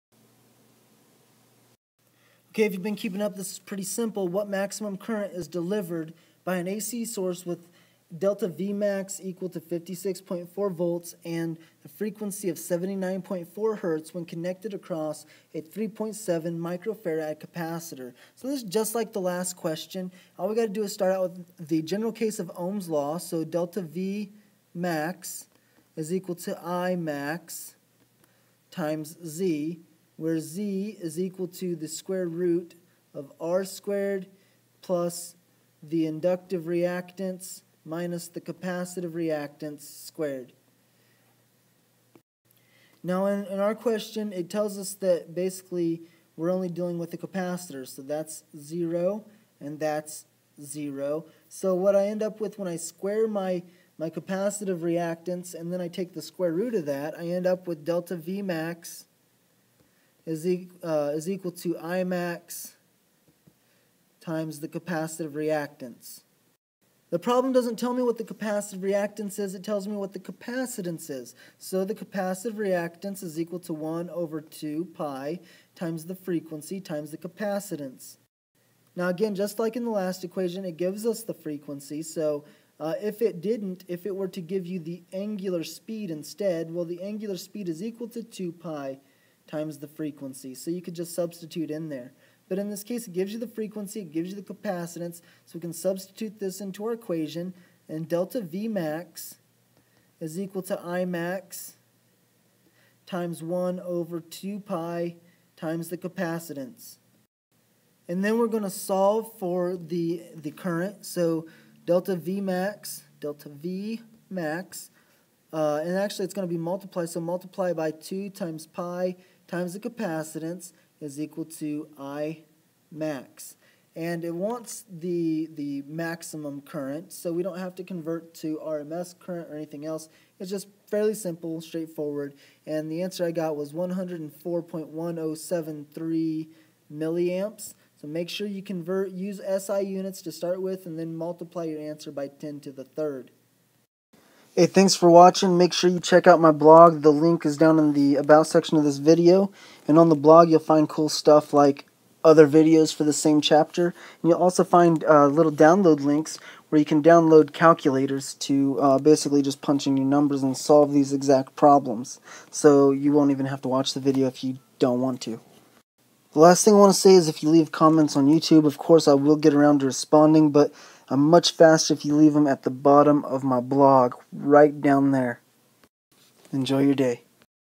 Physics WebAssign Ch21 #4
What maximum current is delivered by an AC source with ΔVmax = 56.4 V and f = 79.4 Hz when connected across a 3.70-µF capacitor?
_____mA